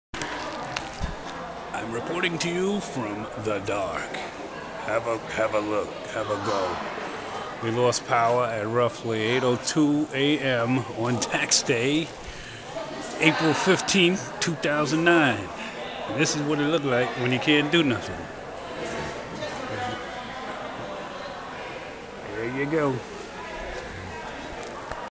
2hr plant blackout short 04 15 09 8am
Here's a breif clip depicting an unfortunate power outage that affected my workplace on April 15th 2009. At approximately 8:02AM, we lost all AC power and were forced to  "sit it out" until power was restored roughly 2 hours later that day